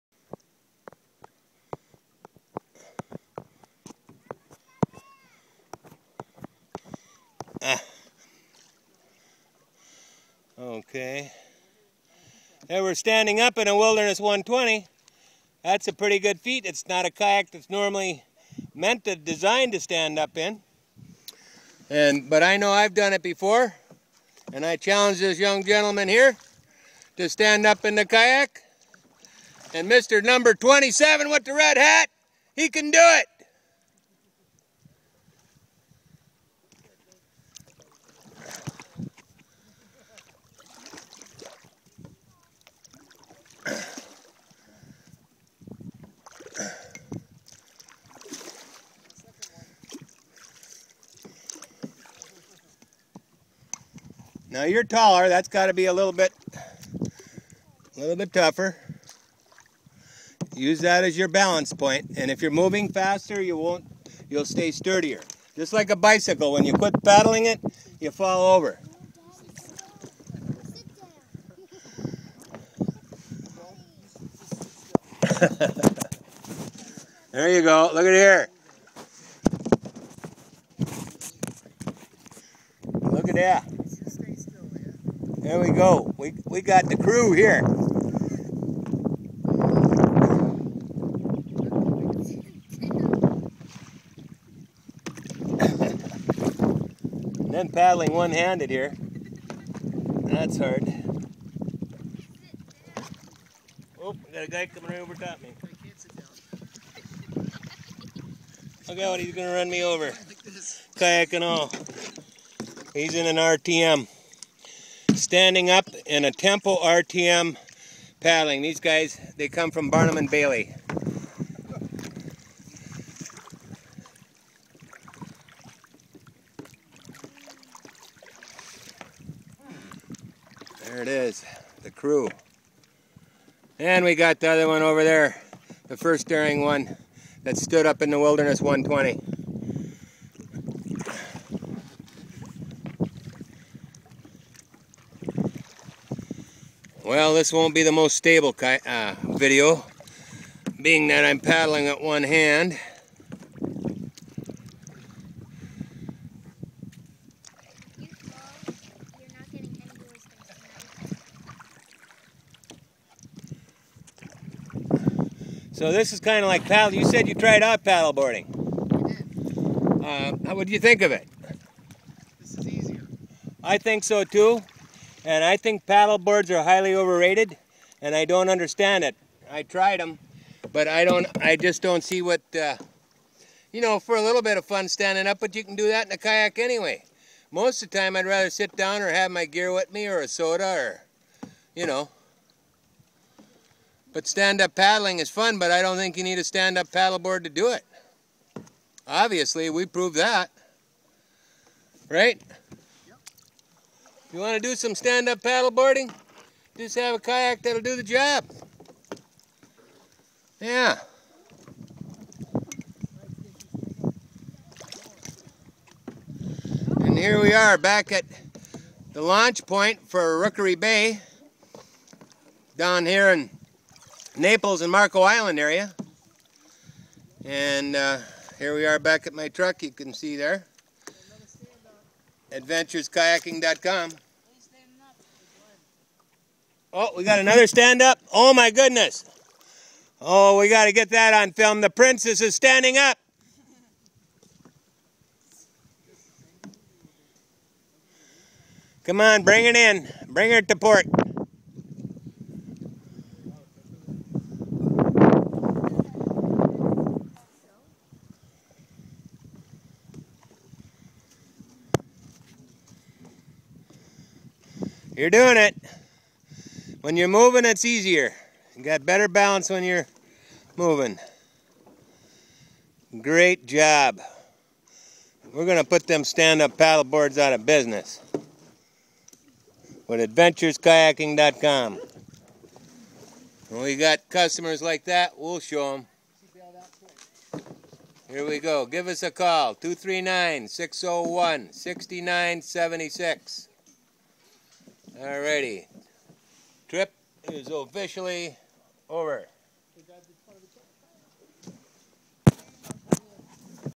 Rookery Bay, Napless Florid with Adventures Kayaking on tour
A wonderfulness preserve saved for us by a large community effort. The Kayaking and fishing is great and in know for the mangrove island with some times hundreds of birds and many species roosting.
While kayaking these waters Adventures kayaking clients prove kayaking is better than stand up paddle boards    ( SUP ) .